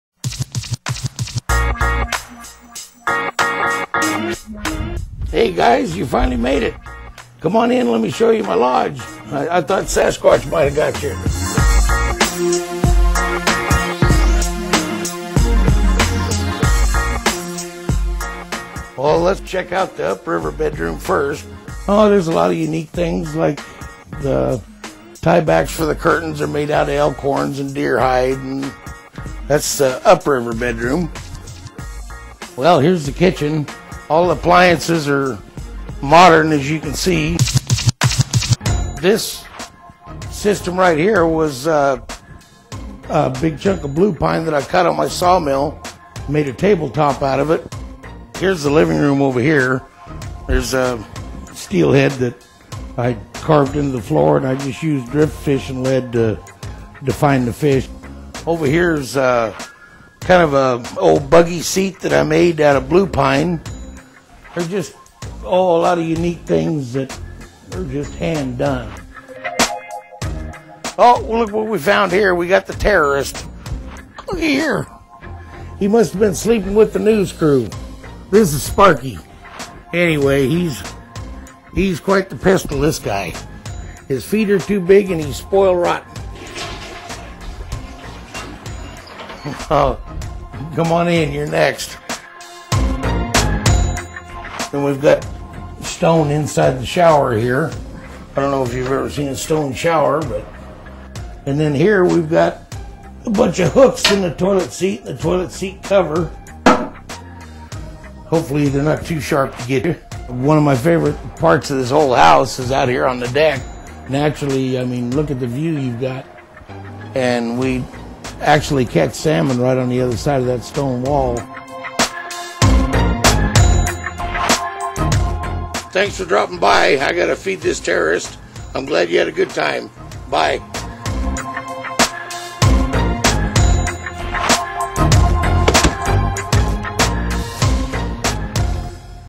Outdoor Storytellers - Lodges
It's time to take a home tour, Outdoor Storyteller-style.  In this edition, it's a lodge with great features and a fantastic view.  Oh, and the assessories fit the place perfectly.